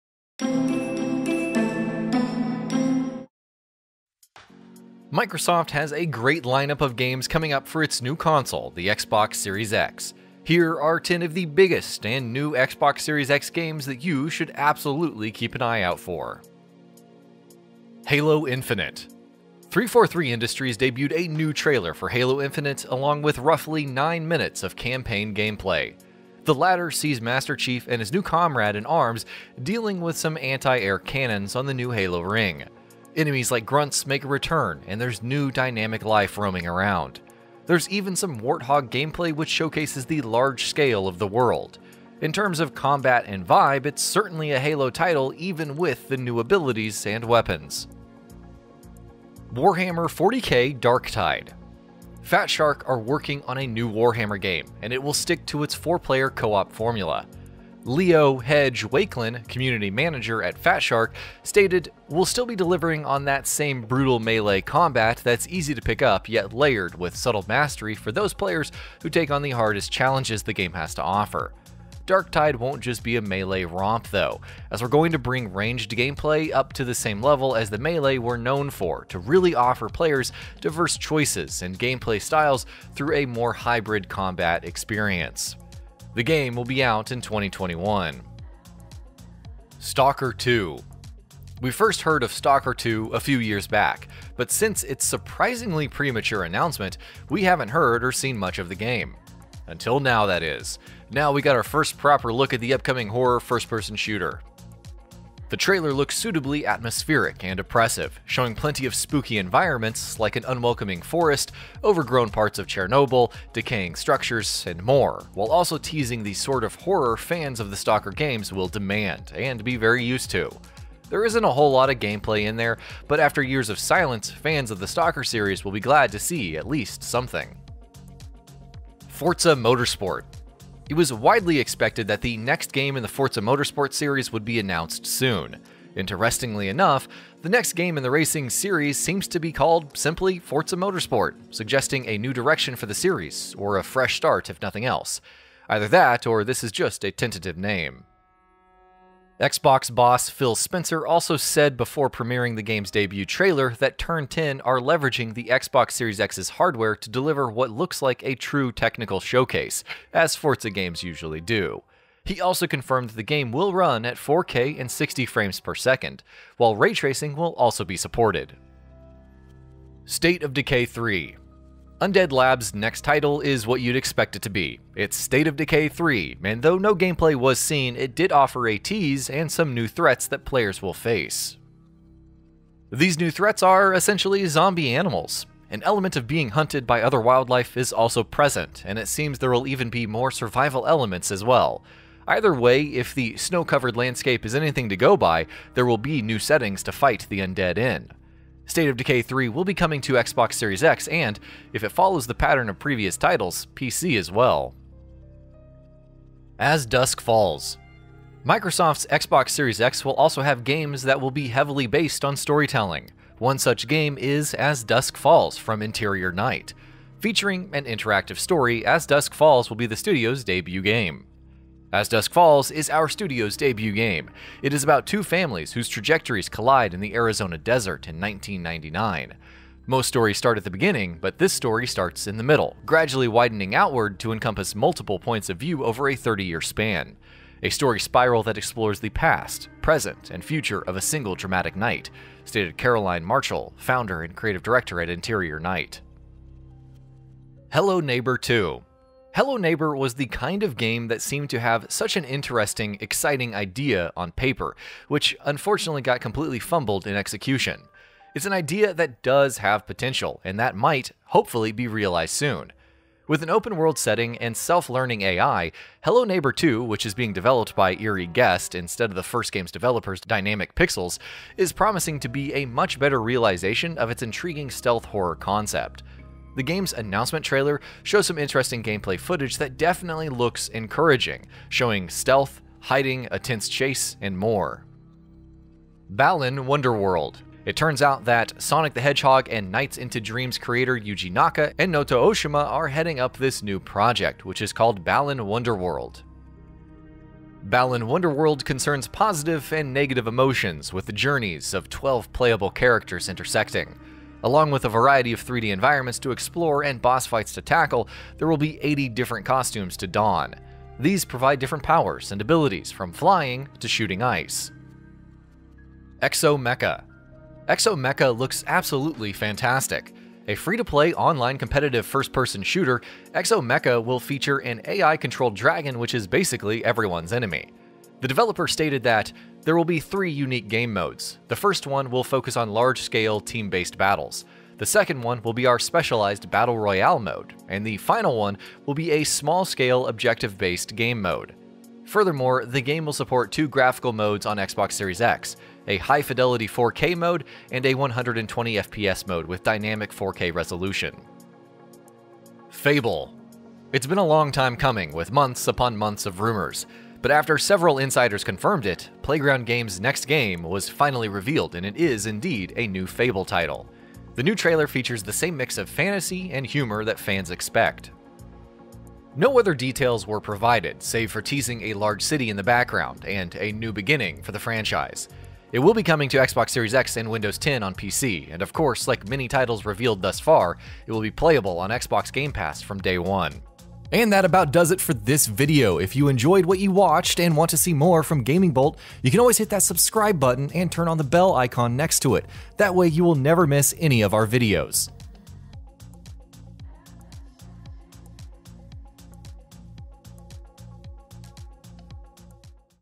10 NEW Xbox Series X Games That BLEW OUR MINDS
Microsoft have a great line up of games coming up for its new console Xbox Series X. Here are the ten of the biggest and new Xbox Series X games you should absolutely keep an eye out for.